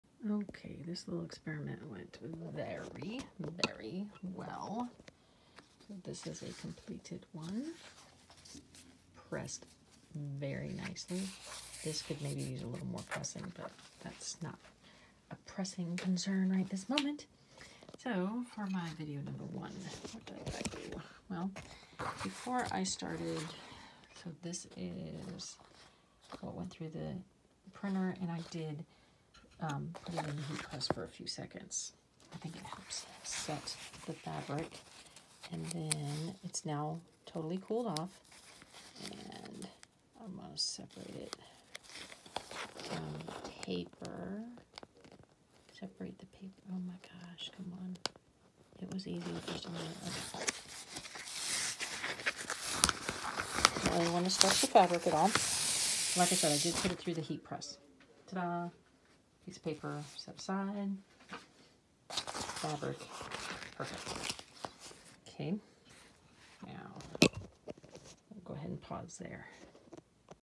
Wedding placemats 1, so sew easy
Back in 2020 I was hosting a wedding in my backyard. It was small and I decided custom placemats would make great guest gifts. The bride is an amazing artist so her drawings were used for each one.
   We also made the food, decorations and I made a wedding quilt…. So I got pretty busy. I asked a friend to help me with the placemats. Here is the instructional video series I made for her.
   I’m calling it ‘wedding placemats’ but you may recognize it as something else….. super cool and easy way to frame a picture on fabric.
  The videography is very terrible and I was embarrassed to be talking to myself so turn your volume up and take a motion pill😜😂🤣